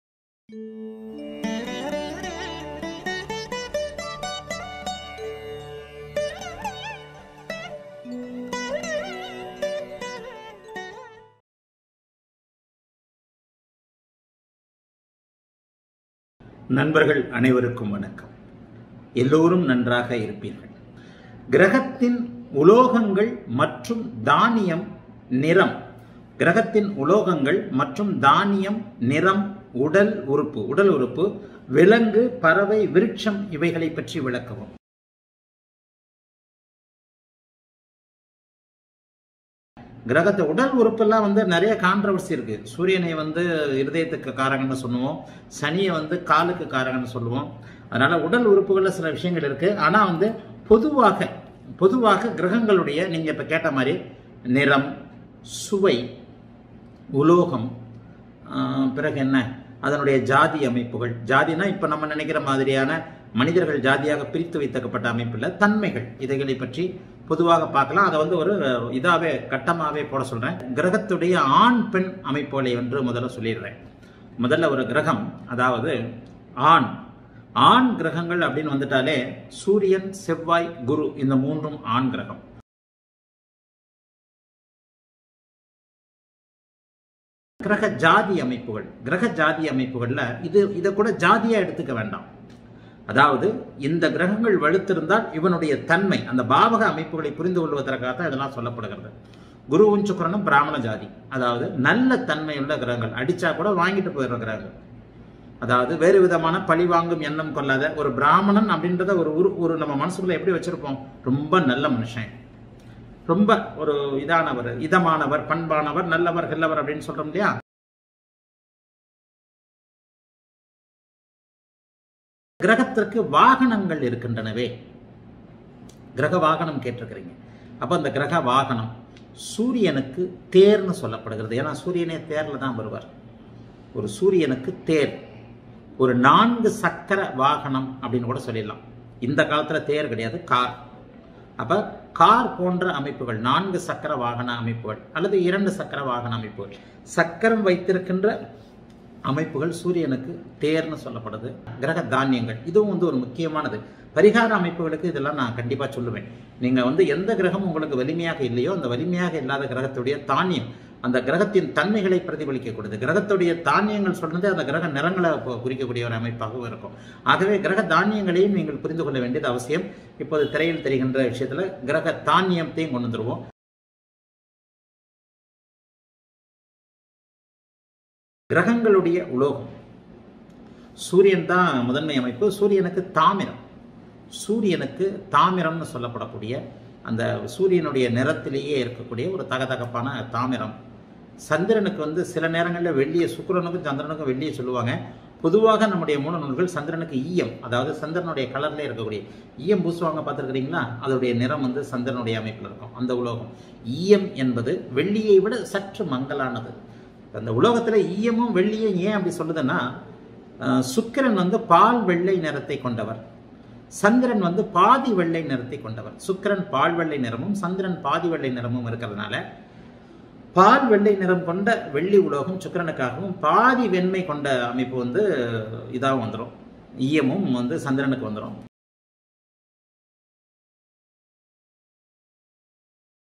1287 - PLANETS COLOUR,VECHILE,METAL,FLOWER DETAILS - TRAILER - 217#adityaguruji
கிரகங்களின் உலோகம், வாகனம் ,நிறம், தான்யம் ,பூ ,தன்மை போன்றவைகளின் விபரங்கள்  ஜோதிடக்கலை அரசு ஆதித்ய குருஜியின் கட்டண வீடியோ..PREMIUM VIDEO- 217